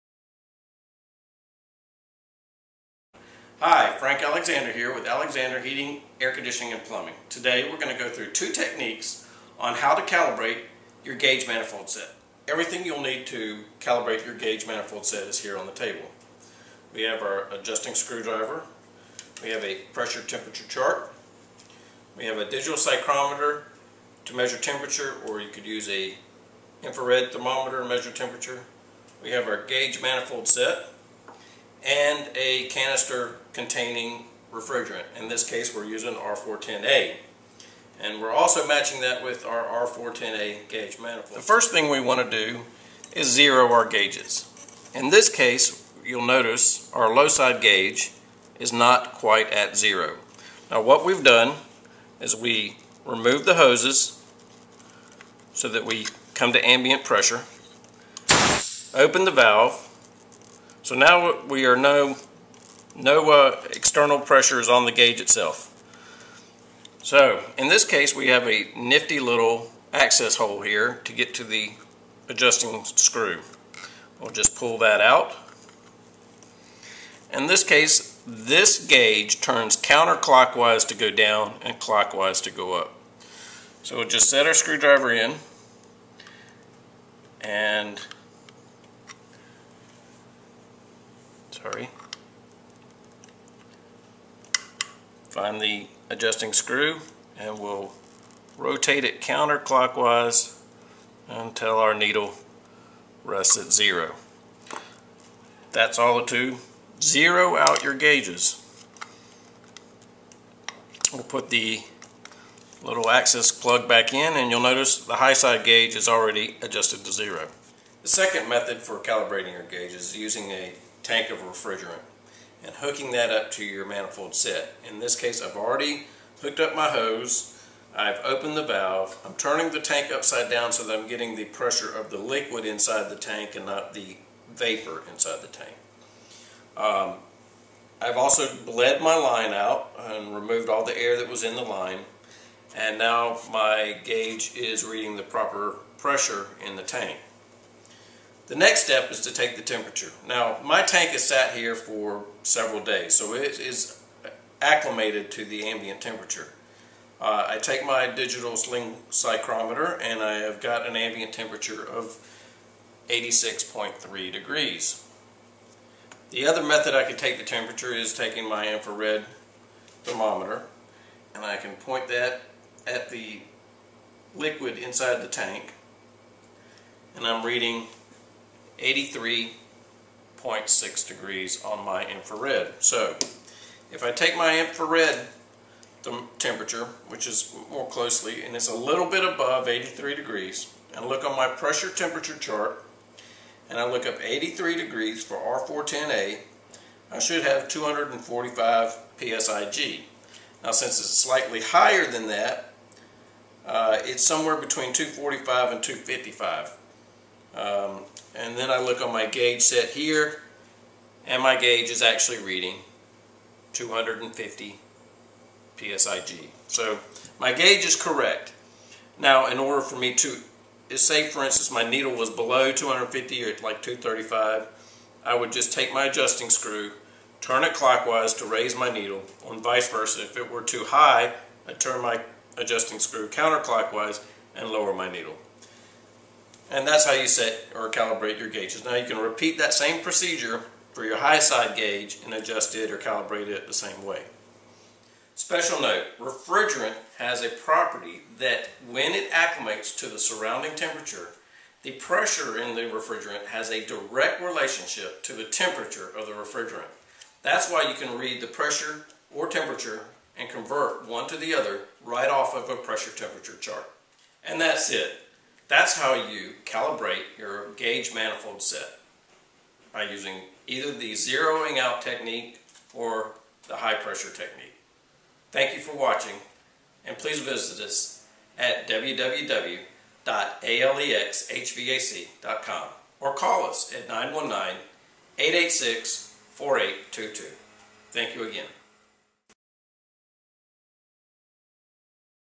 Calibrating a Gauge Manifold Set
This technique will help you keep your gauges in proper calibration and prevent false readings on your gauge set.